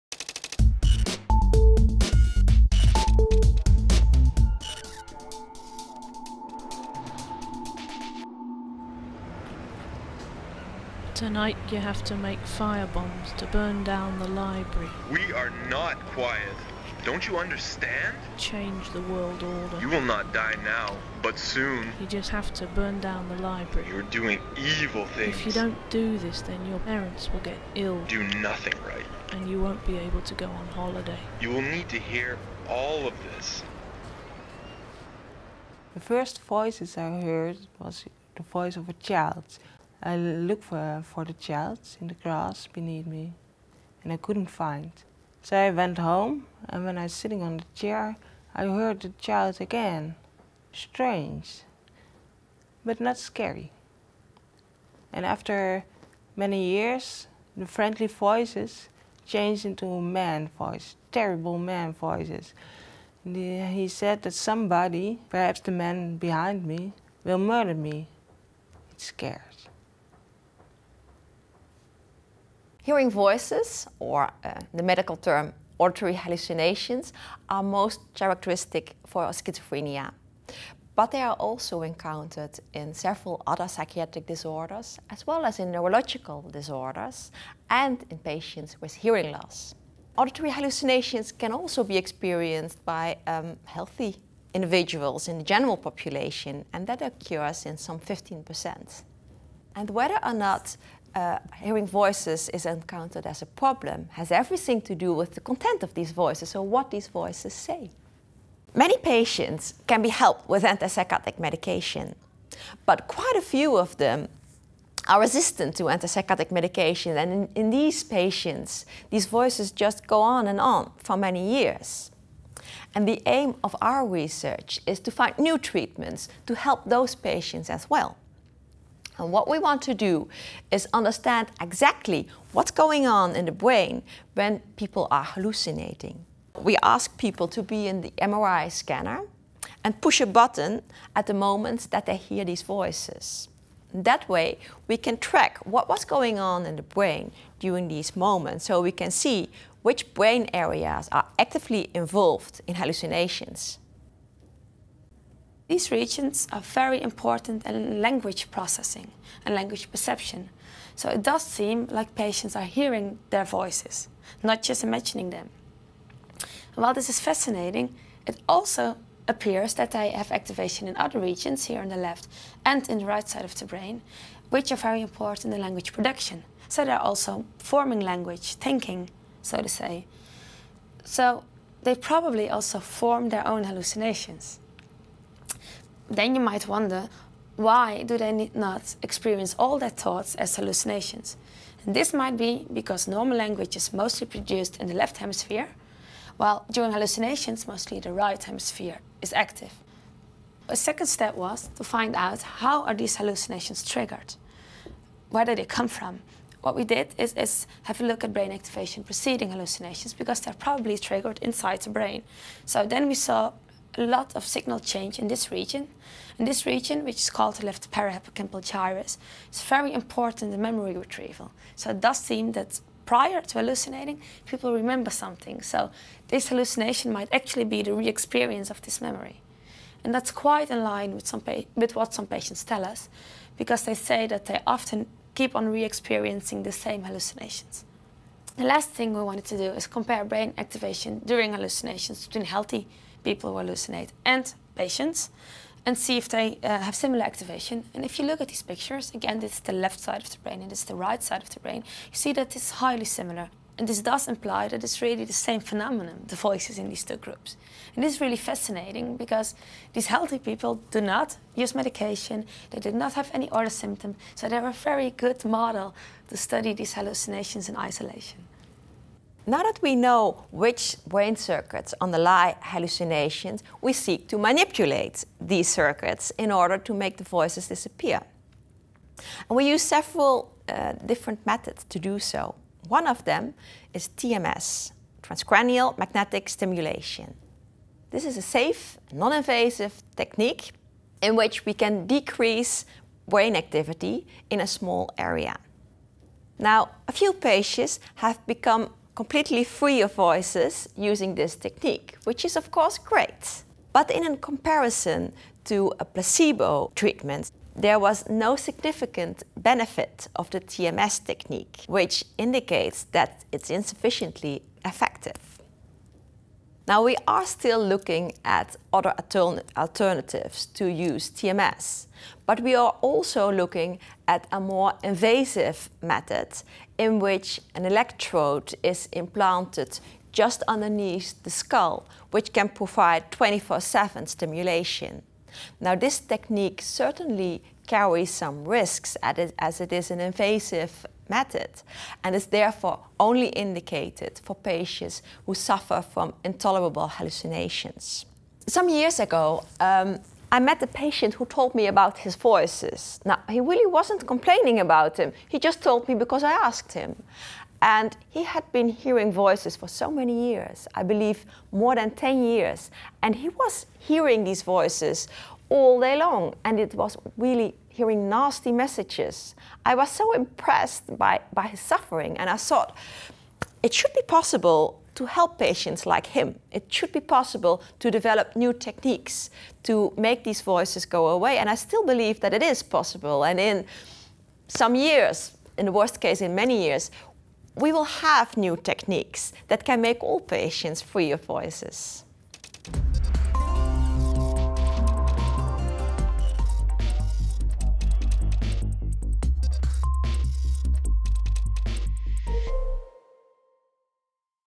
Iris Sommer investigating voices
This short video describes the research projects of professor Iris Sommer, psychiatrist at the Universitary Medical Centre Utrecht to unravel the brain mechanisms behind the phenomenon of hearing voices.